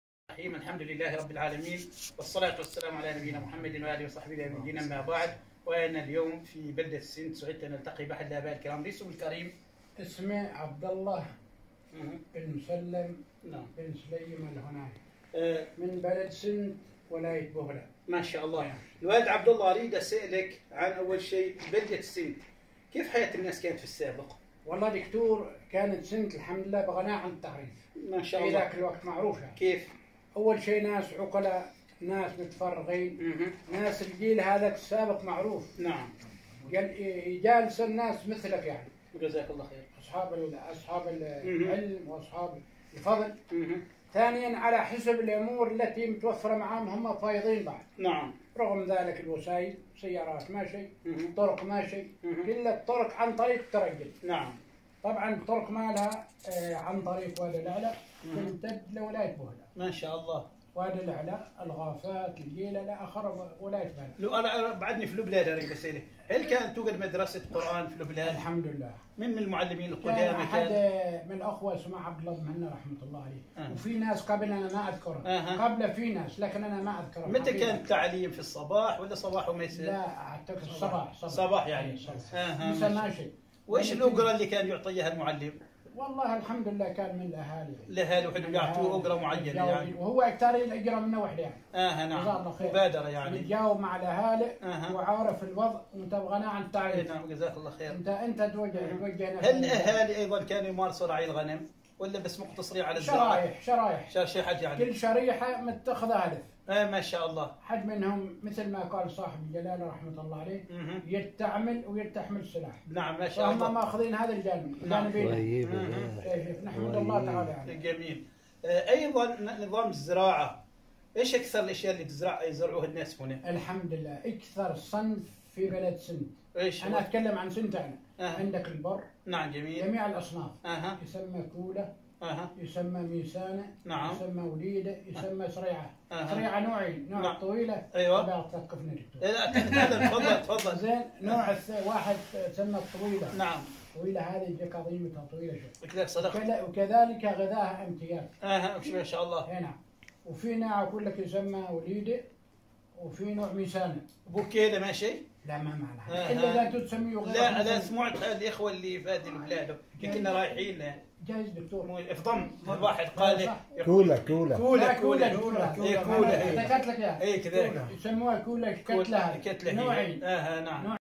الحياة الإجتماعية في بلدة سنت، حديث مع الوالد عبدالله بن مسلم الهنائي.
الأعمال الزراعية.